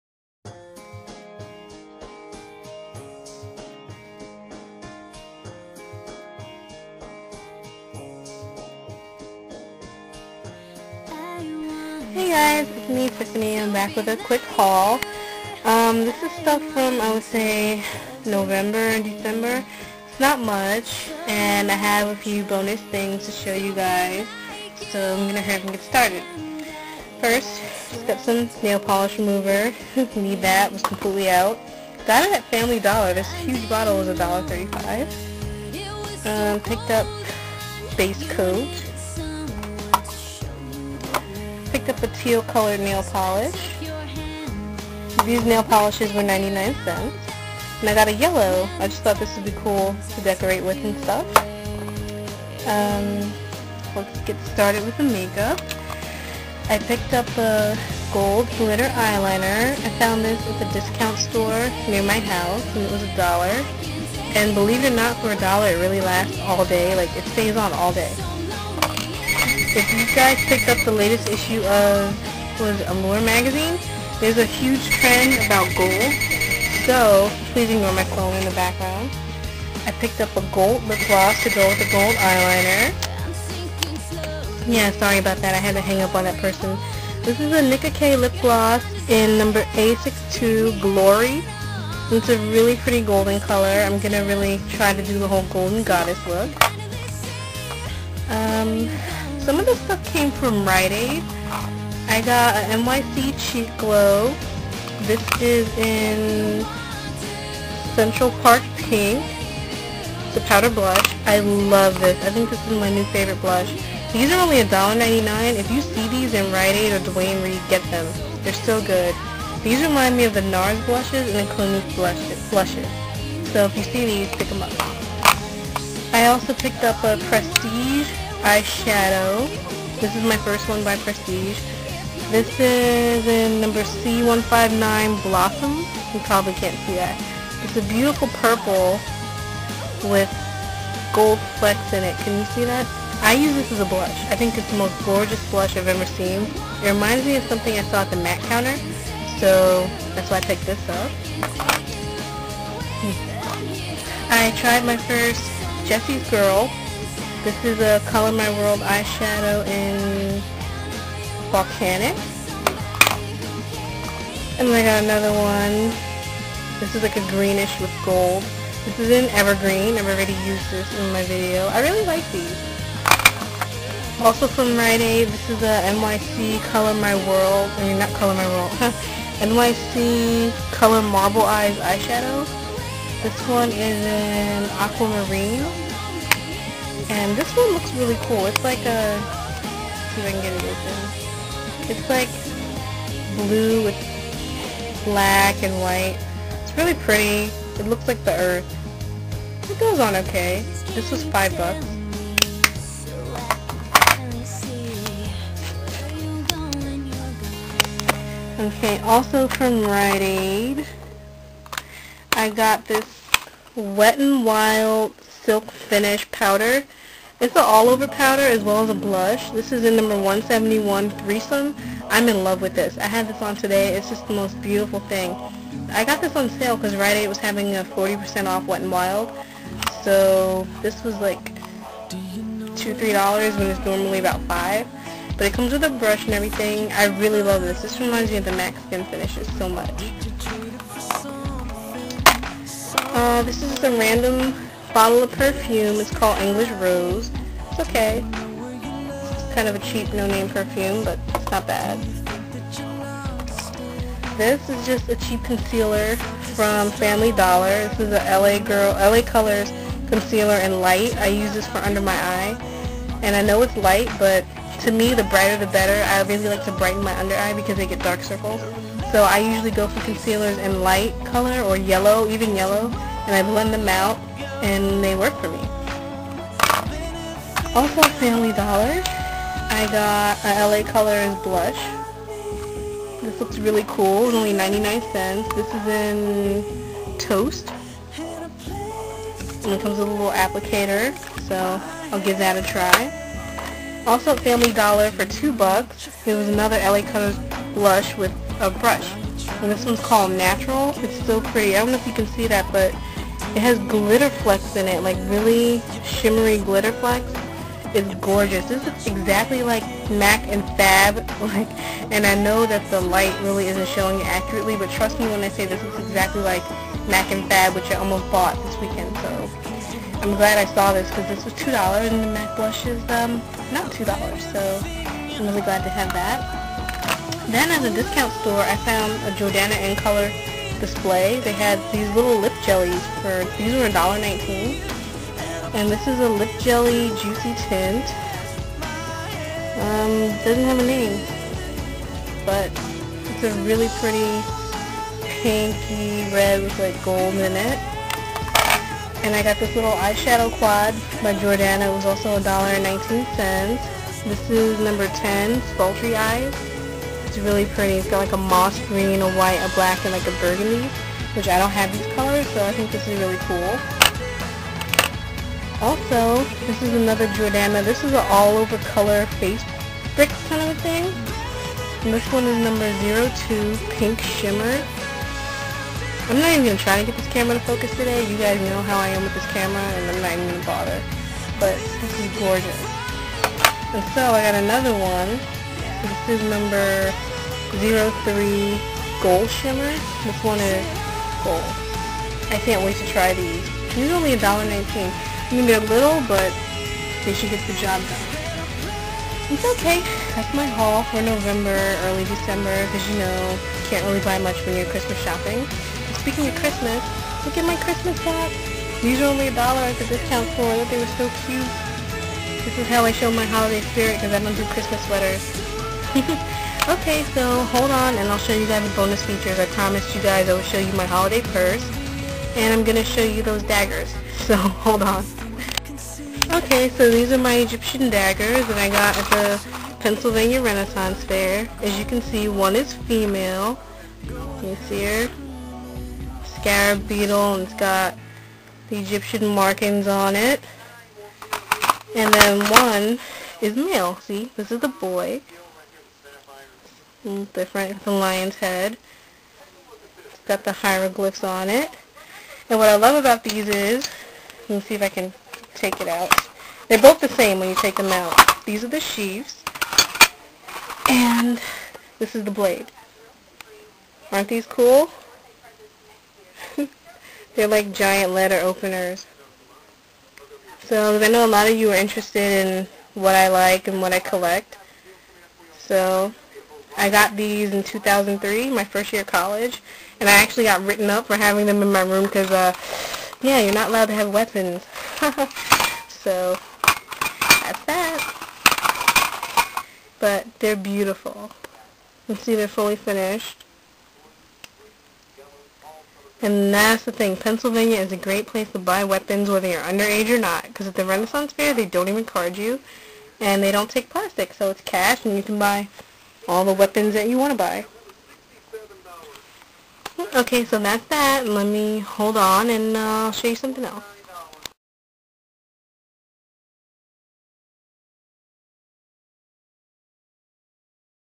Nov/Dec makeup Haul + cool stuff
This is a makeup haul plus my egyptian daggers lol enjoy